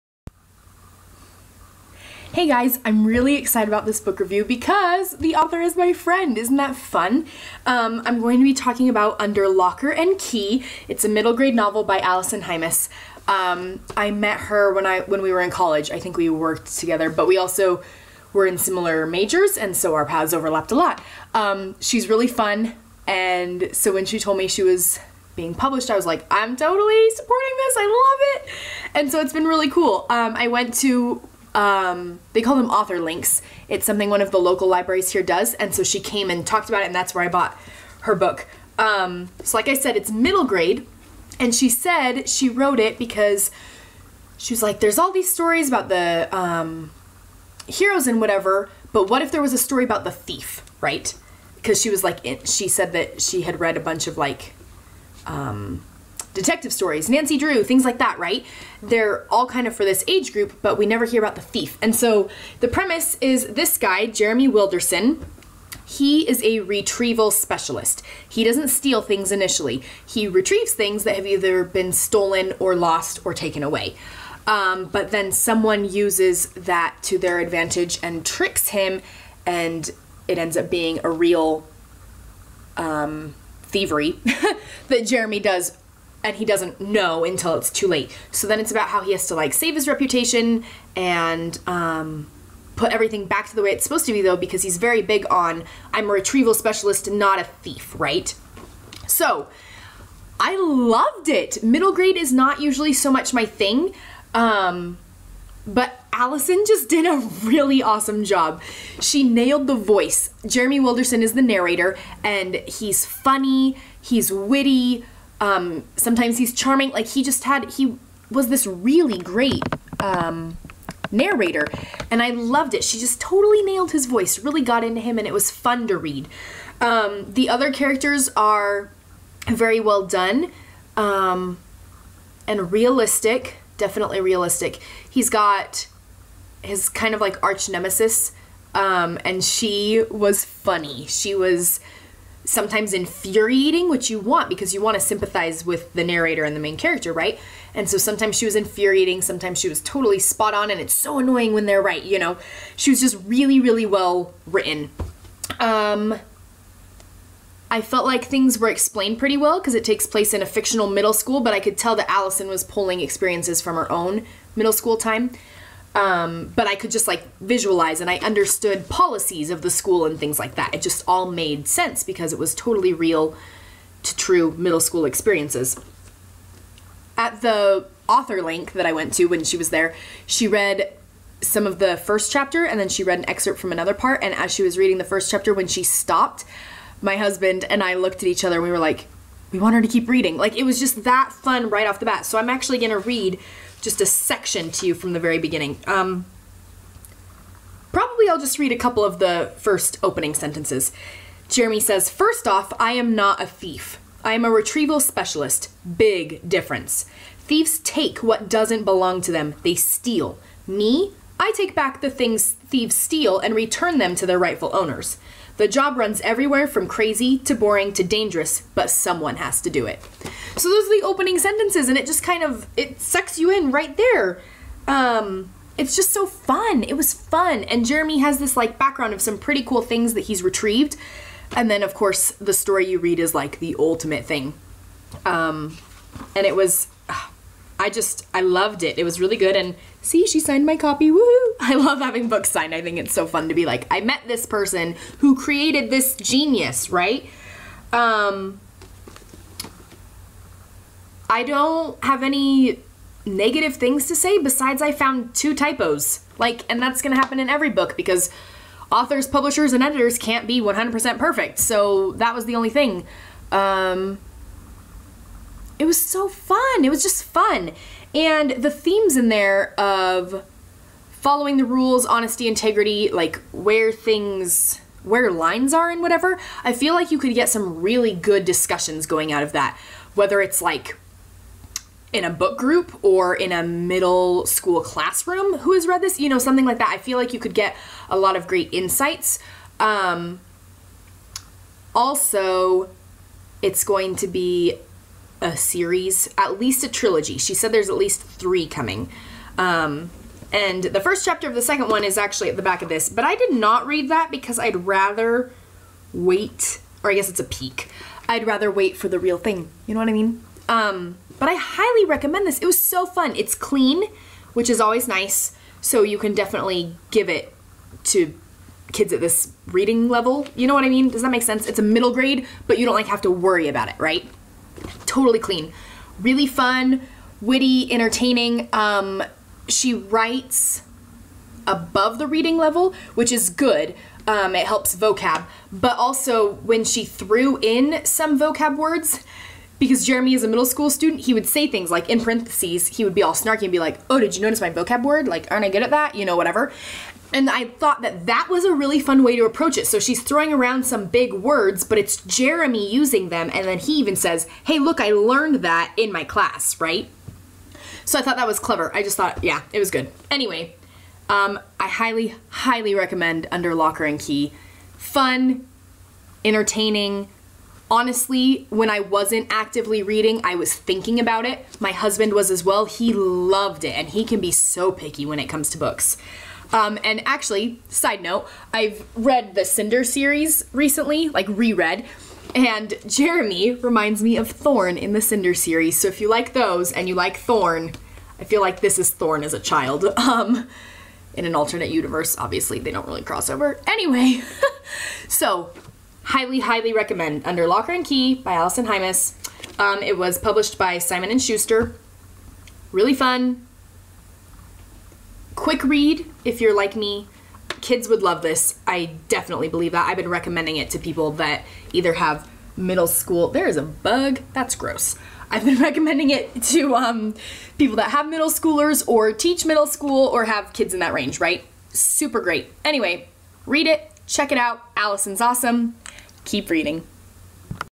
Middle Grade Win
A book review of Under Locker and Key by Allison K. Hymas.